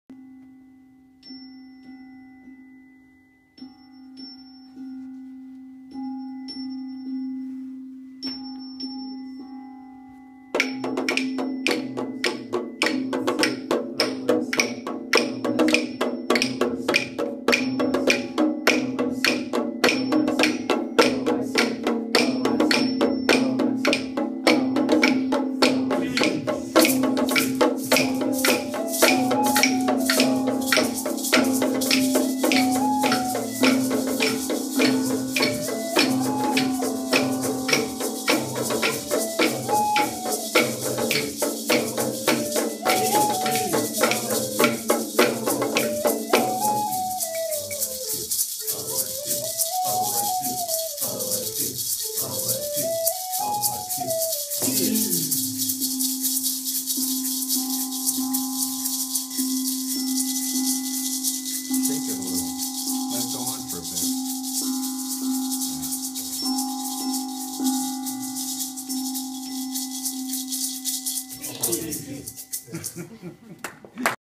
HC 407 Soundscaping, Performance 2
Students in Tom Strini's Soundscaping colloquium for the University Honors College at Oregon State University created scores of music with creative and non-traditional methods. This is one of the performances at the end of the class.